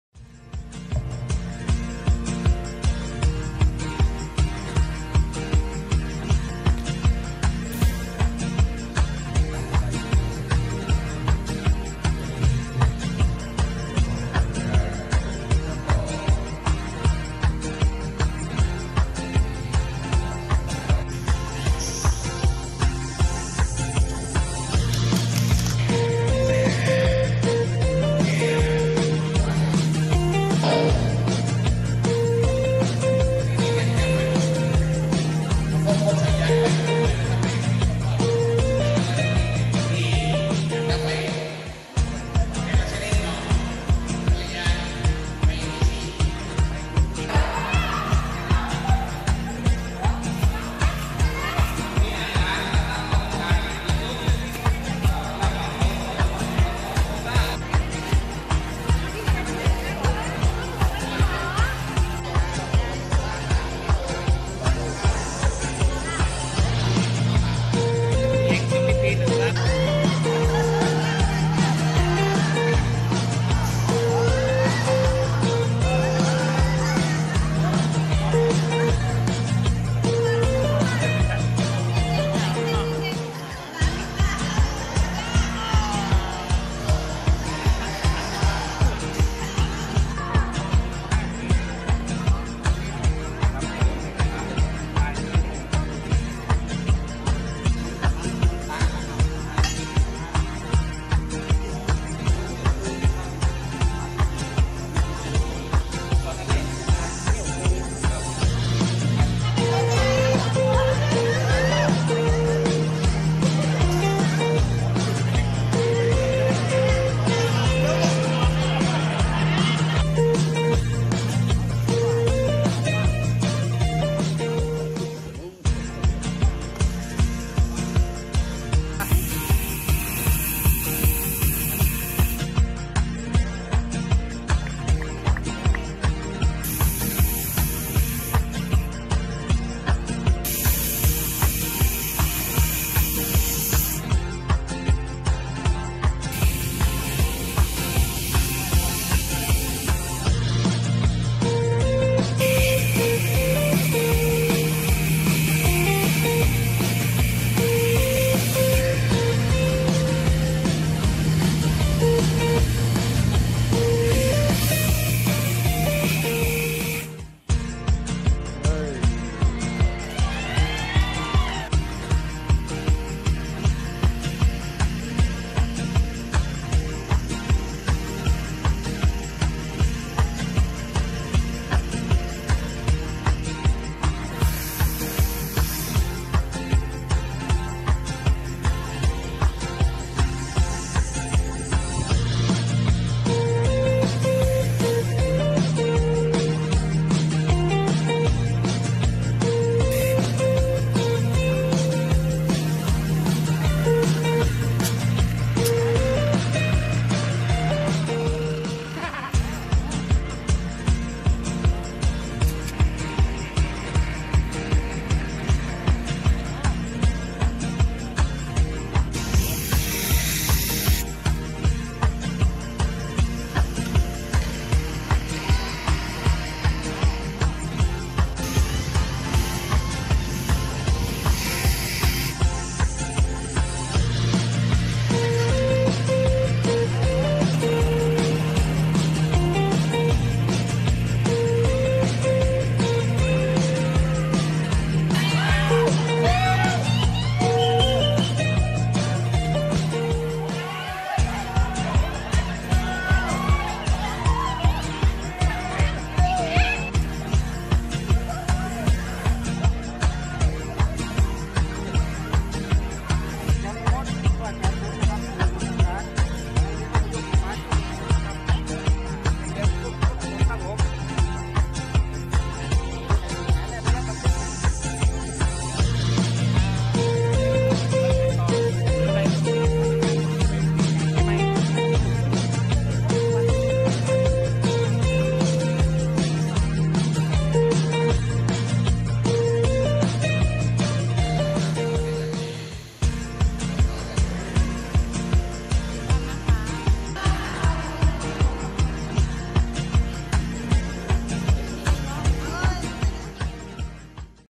การฝึกอบรมดับเพลิงขั้นต้น โรงเรียนน่านปัญญานุกูล จังหวัดน่าน
เมื่อวันพุธ ที่ 24 กรกฎาคม พ.ศ.2562
การฝึกอบรมดับเพลิงขั้นต้น
ฝึกซ้อมดับเพลิง และฝึกซ้อมอพยพหนีไฟ
โรงเรียนน่านปัญญานุกูล จังหวัดน่าน